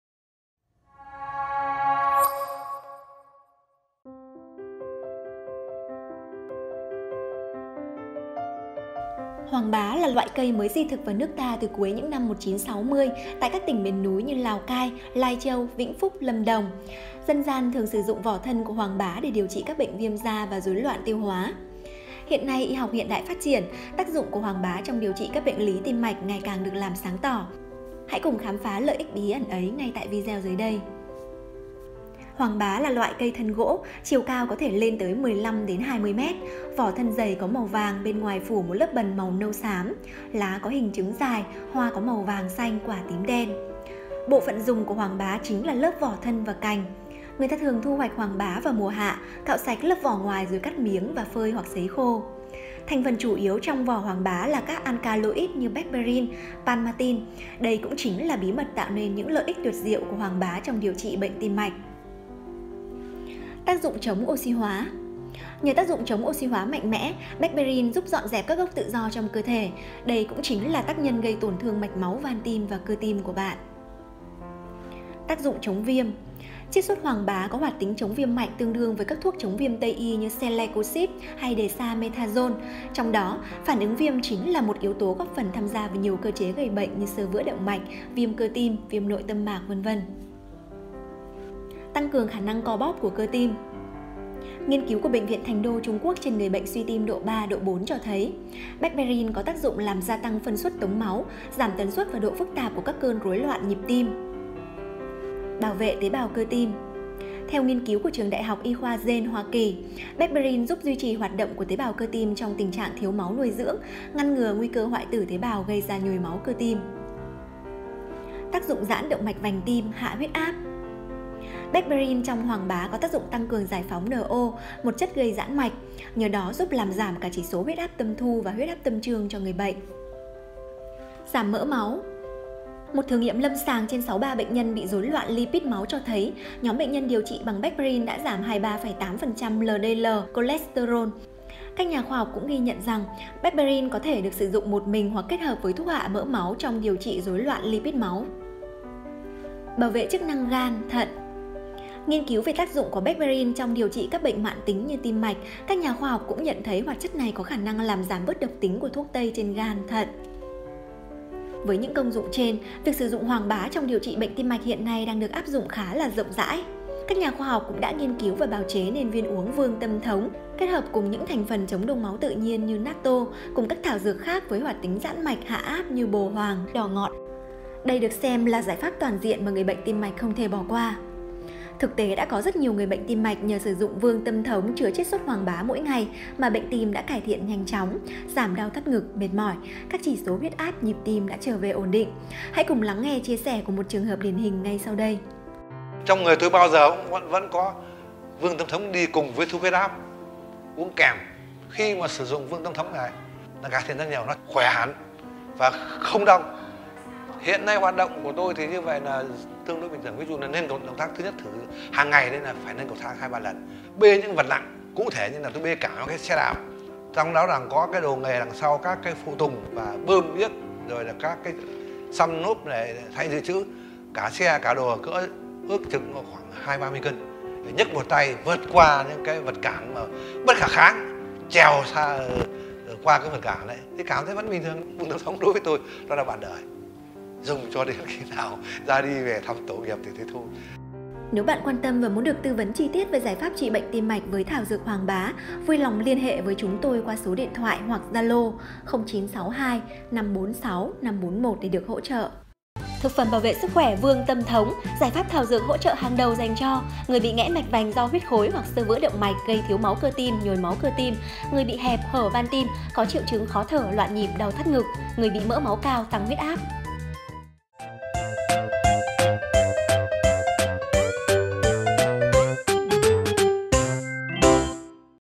Hoàng bá - Cây thuốc quý cho người bệnh tim mạch
Hoàng bá là loại cây mới di thực vào nước ta từ cuối những năm 1960 tại các tỉnh miền núi như Lào Cai, Lai Châu, Vĩnh Phúc, Lâm Đồng. Người ta thường sử dụng vỏ thân của loài cây này để điều các bệnh viêm da, lở loét và rối loạn tiêu hóa.
Ngày nay, y học hiện đại phát triển, những lợi ích của Hoàng bá đối với sức khỏe tim mạch ngày cảng được sáng tỏ. Hãy cùng khám phá bí ẩn đằng sau vị thảo dược này mà rất ít người biết tới tại video này.